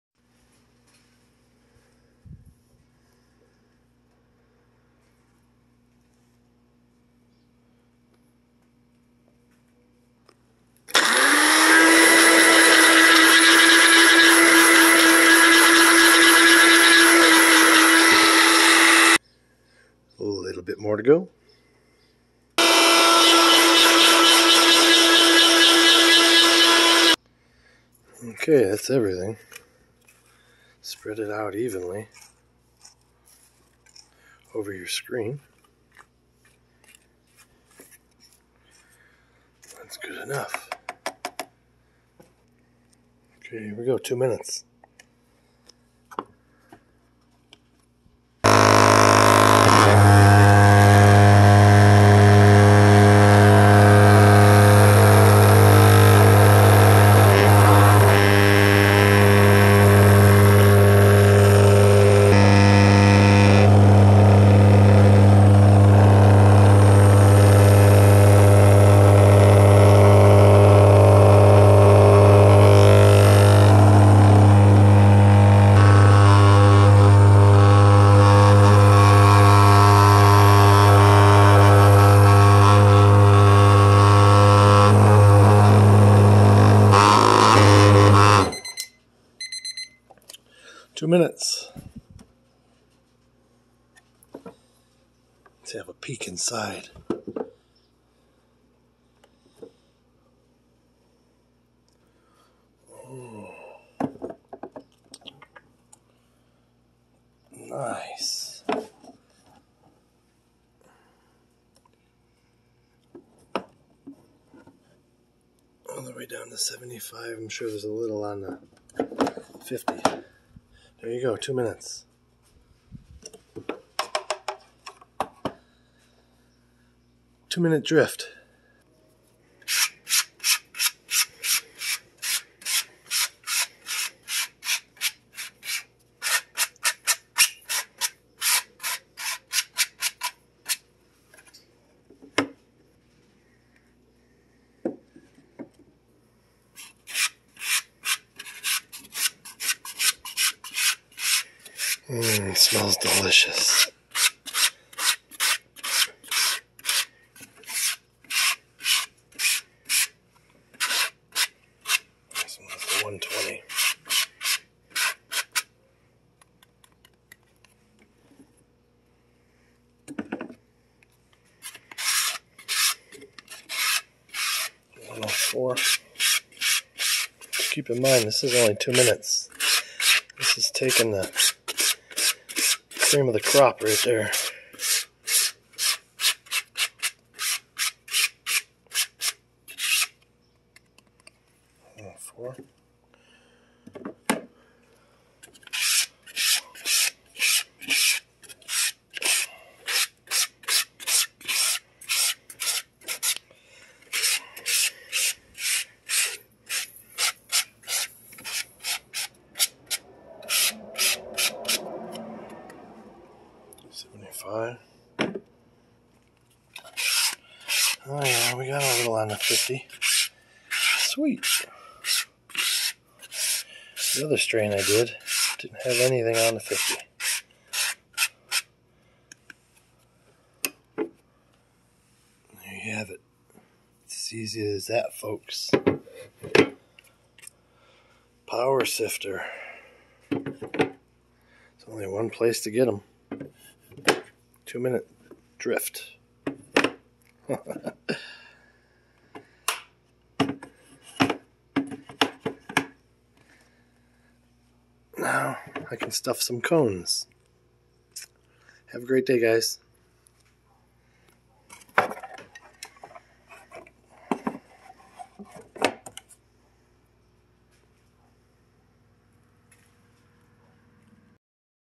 HK's MySifter 12 x12- 6 Screen POWER SIFTER in *** ACTION!***
6 Stainless Steel Screens of DRIFTING POWER!   Fill up your Sifter's Top Work Screen with ground up Dryed Materials...plug MySifter into a 110v wall outlet...turn knob to HIGH-VIB...! 2 MINUTE DRIFT is all i suggest per batch...of course...you can and thwre are people that will vibrate EVERYTHING thru the screens! THIS IS NOT WHAT YOU WANT TO DO...for The Best of the BEST ! While your MySifter is VIBRATING...firmly place your hands on the lid...as to evenly disperse the vibrating pressure evenly downward...but not too hard to totally DAMPEN the vibration...! Trust me...you will quickly get the hang of it! Once done VIBRATING...use a brush or business card to gently scrape the screens into lovely piles of perfectly sifted material! Now...if desired you could run the Vibrating Lid on top of each micron frame  down to the 50 micron vibrating each as you go down!

~Hashman out...